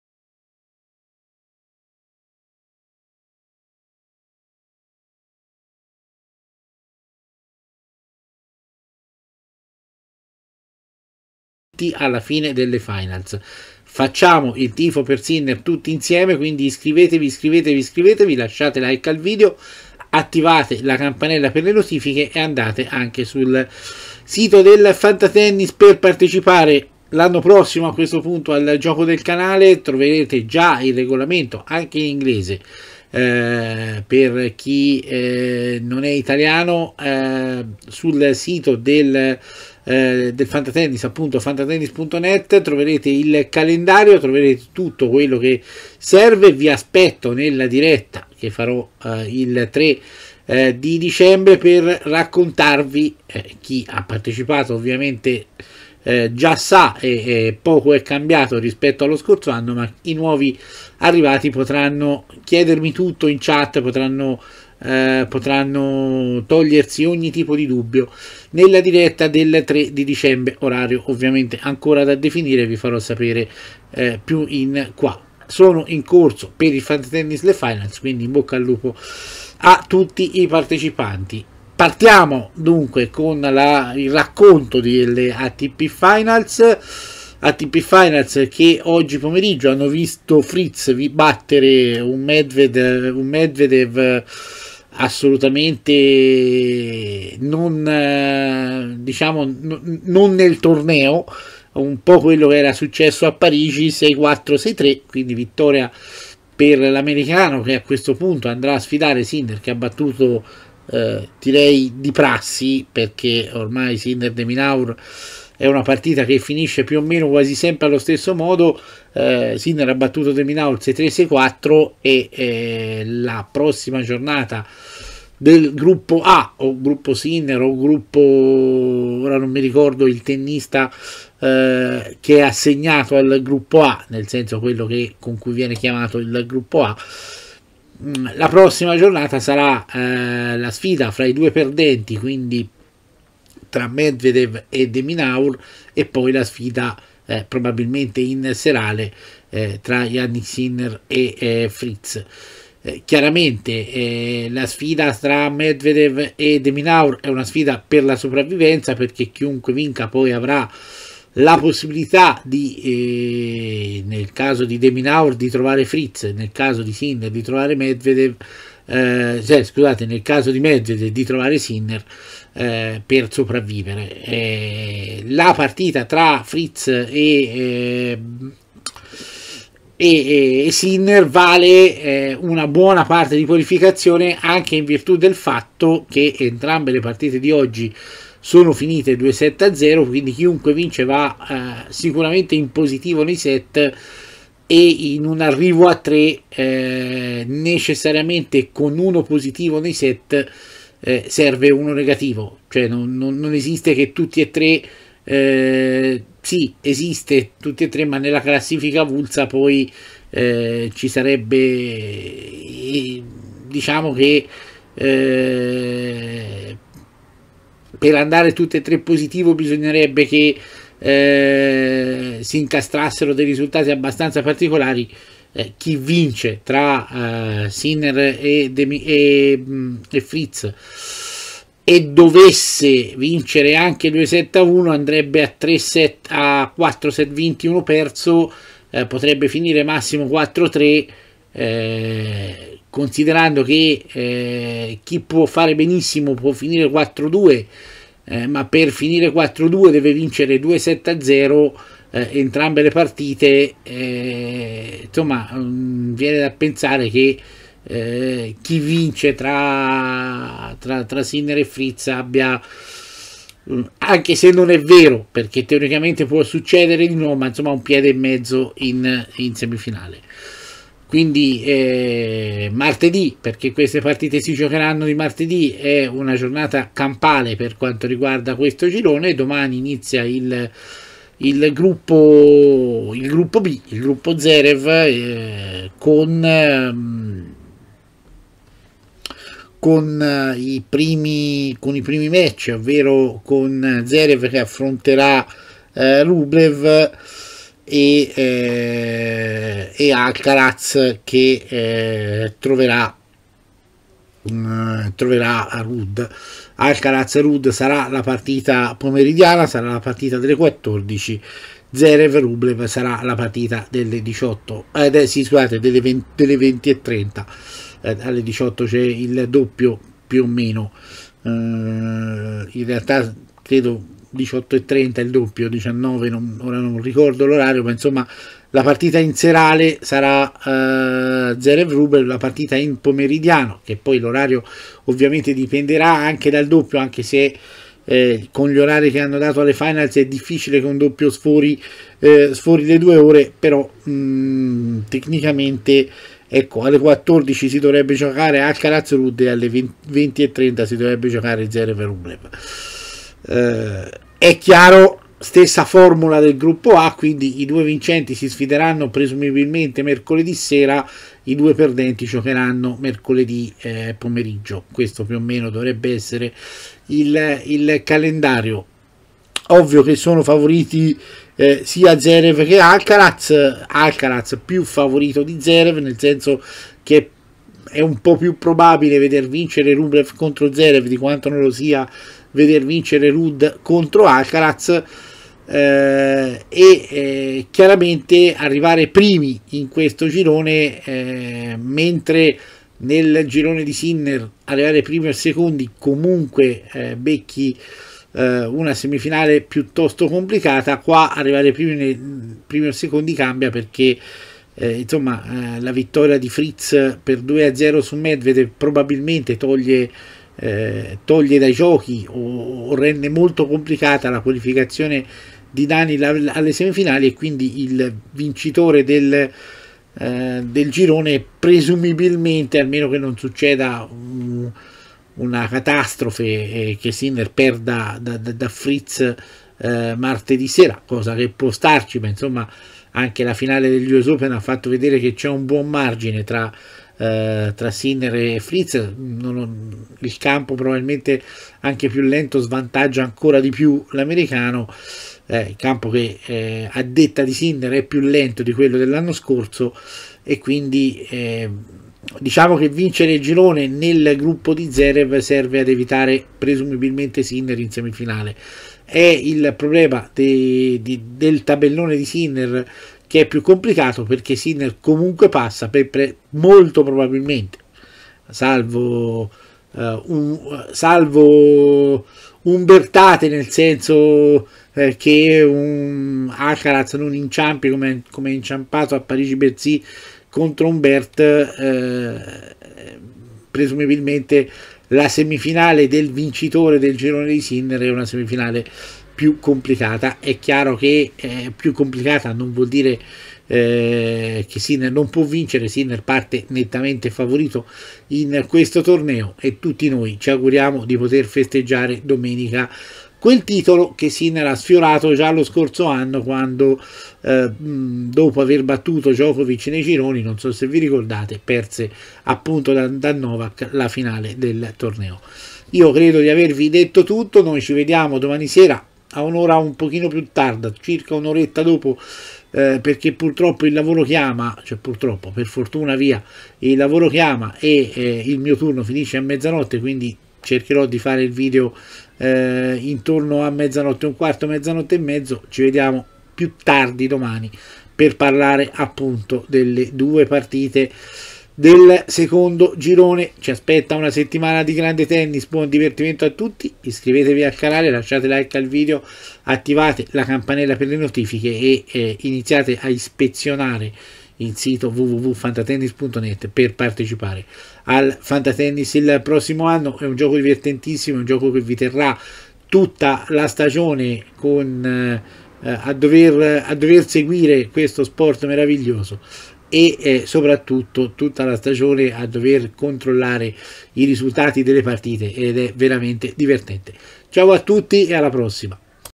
ATP FINALS: Sinner-De Minaur 63 64. Tutto facile per Jannik.
Buona la prima per Jannik che batte in sue set De Minaur. Martedì sfida a Fritz che ha battuto Medvedev sempre in due set.